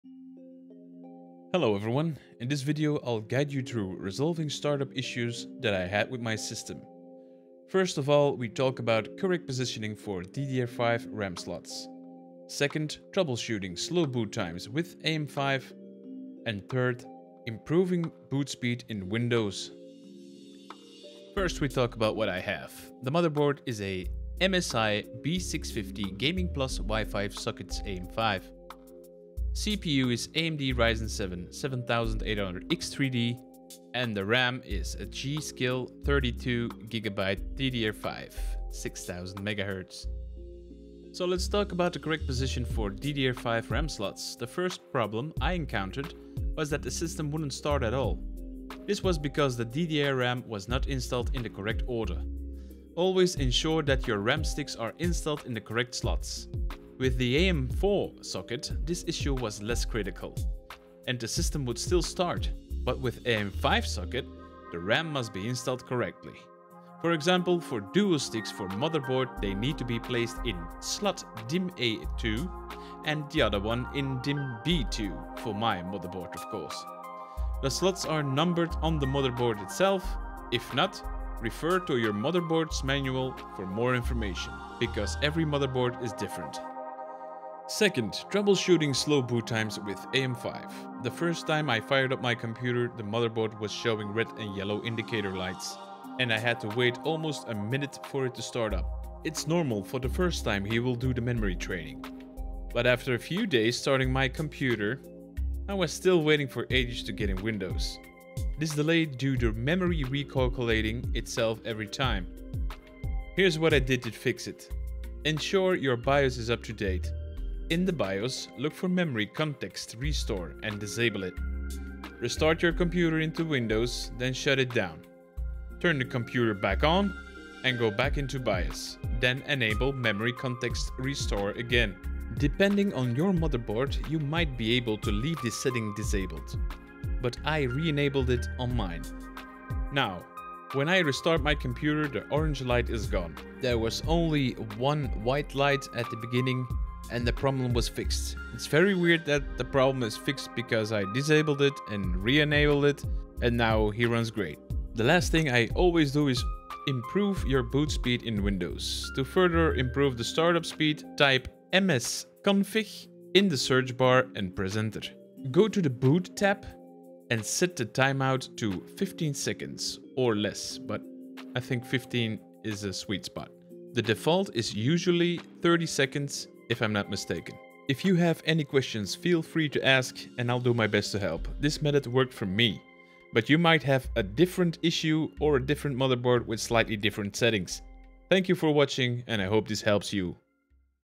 Fix AM5 Boot Issues: Correct DDR5 Installation & Speed Up Startup Time!
00:00 - 00:20 Intro
00:21 - 00:39 System spec
00:40 - 01:27 DDR Position
01:27 - 02:34 Slow Boot (myfix)
02:34 - 02:57 Boot speed

In this video, I’ll show you how to fix boot issues on an AM5 system, specifically with the MSI B650 GAMING PLUS WIFI motherboard and 7800x3D CPU. Learn how to correctly install DDR5 RAM, resolve slow boot times, and optimize startup speed. Follow these steps to ensure your system runs smoothly and efficiently!